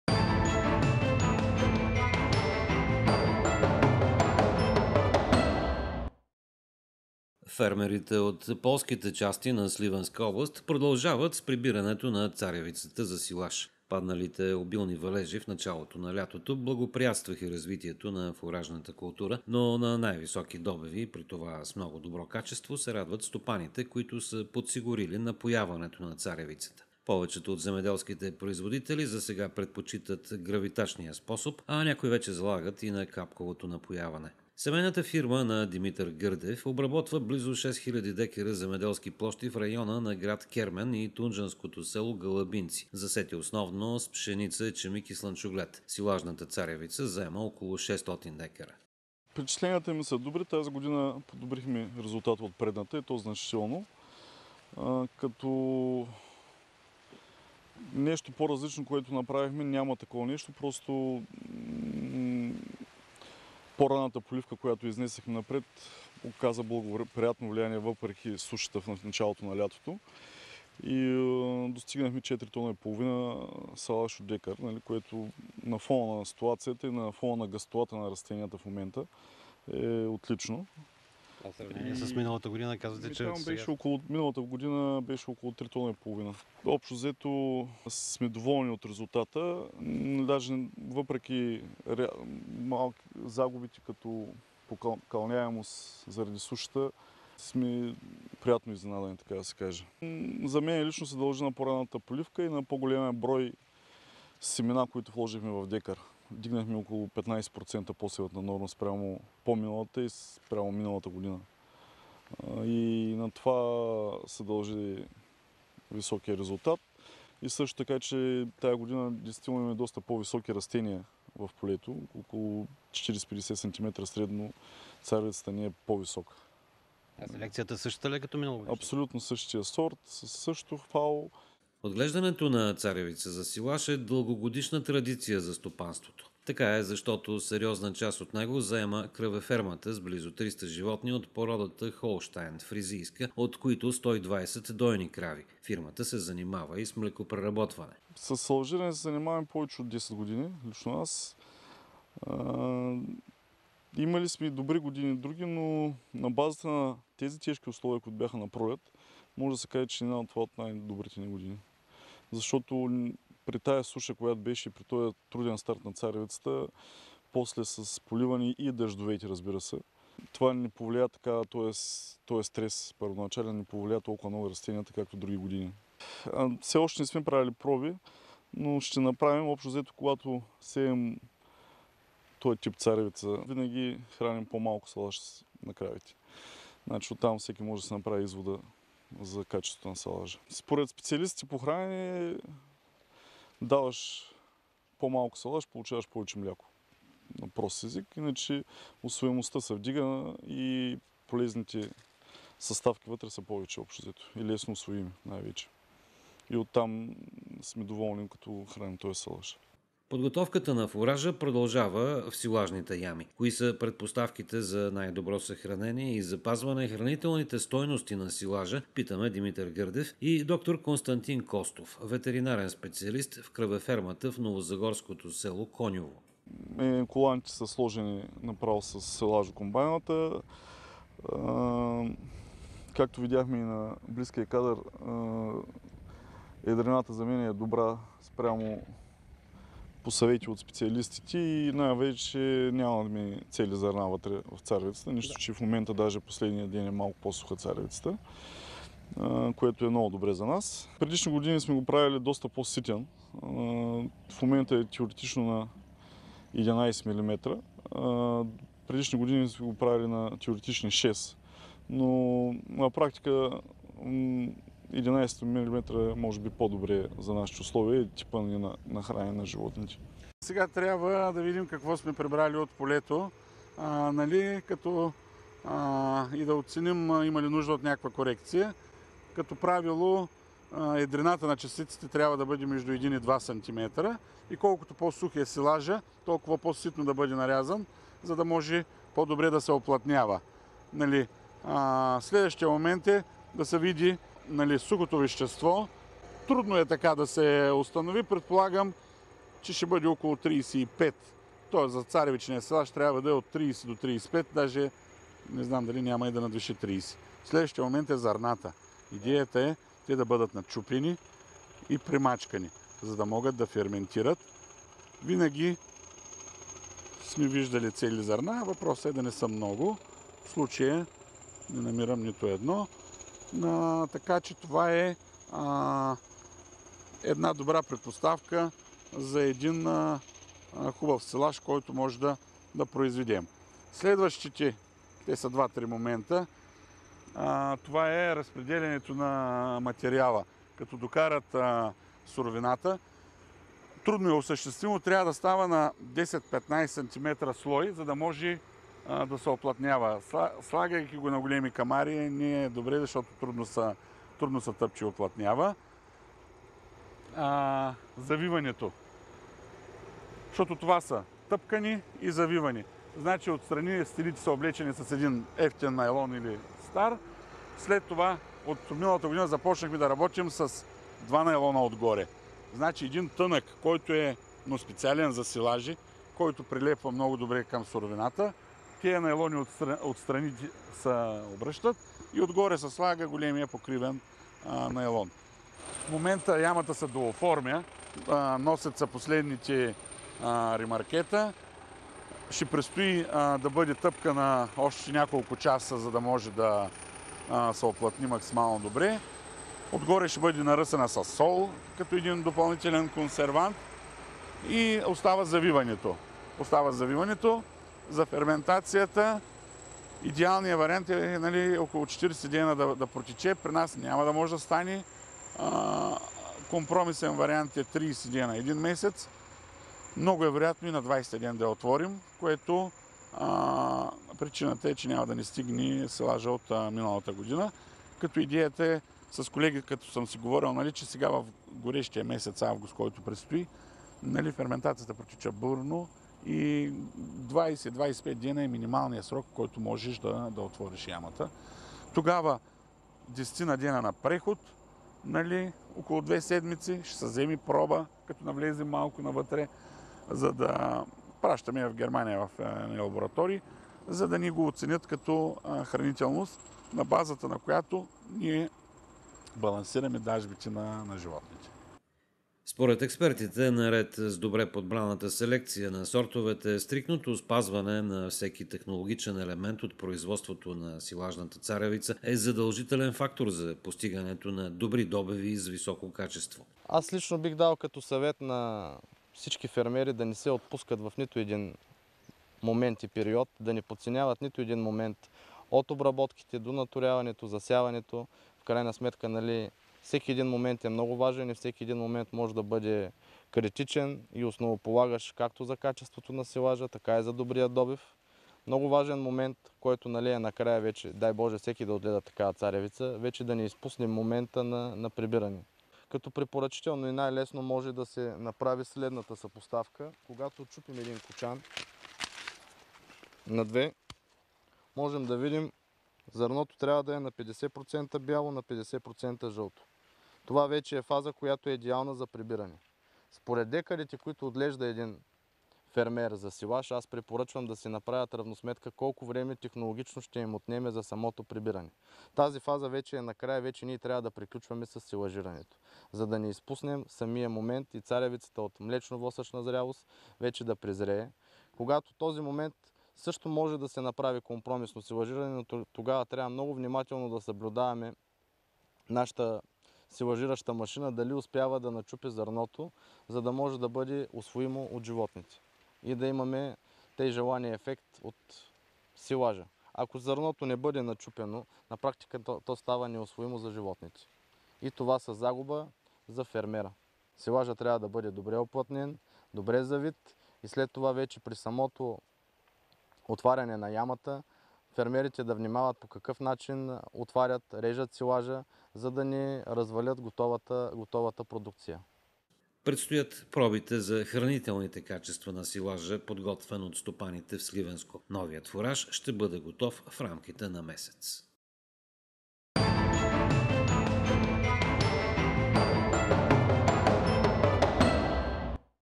Агротема: В Сливенско прибират царевицата за силаж, автор Валентин Вълчев
Добри добиви и високо качество